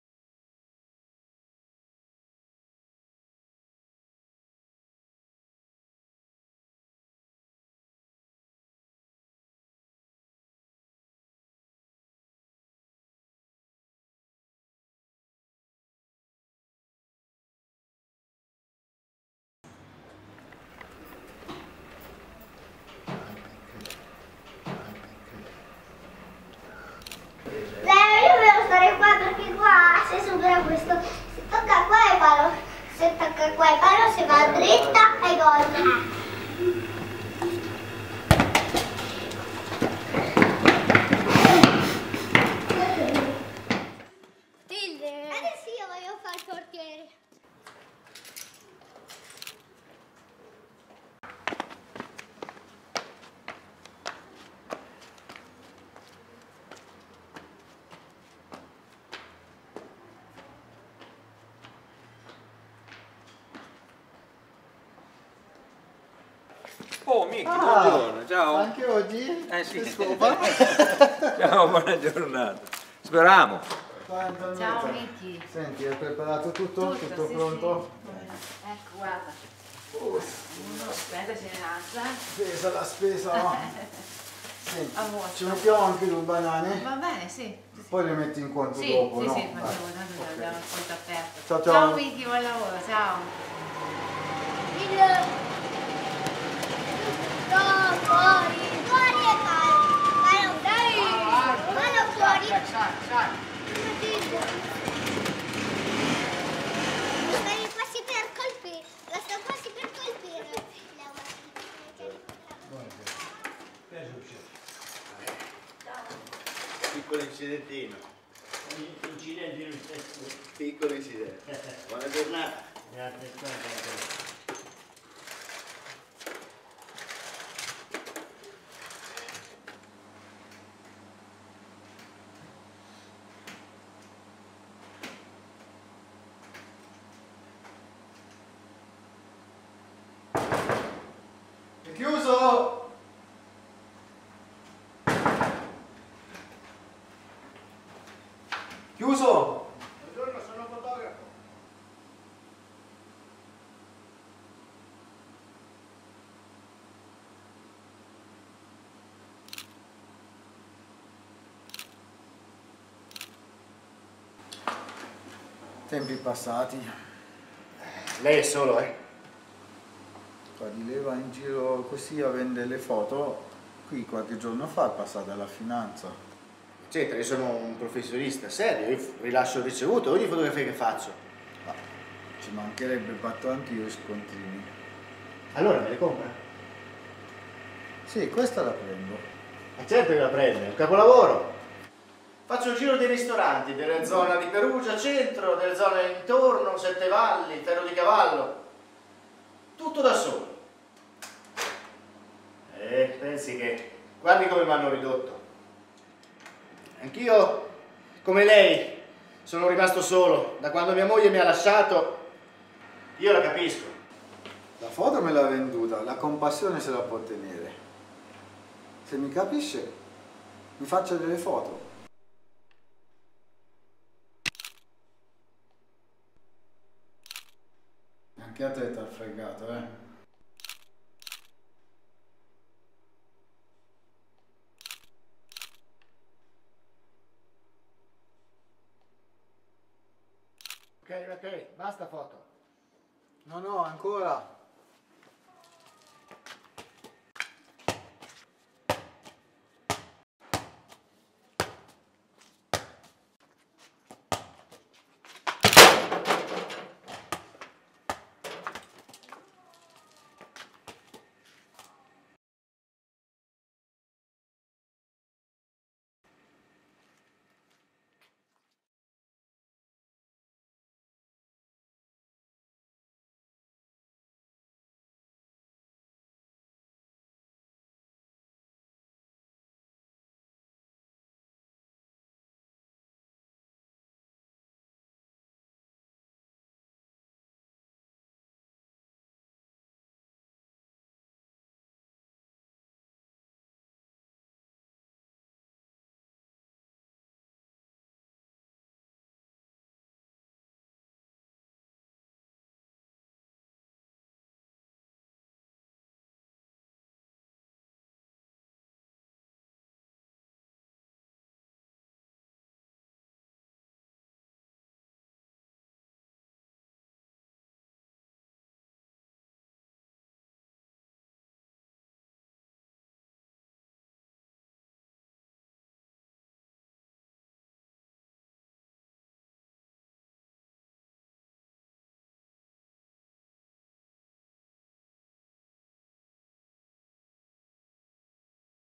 Mirino (Sguardi su Perugia)
cortometraggio-saggio alla fine dei primi 9 mesi di corso di cinema.
Laboratorio di Cinema Gabriele Anastasio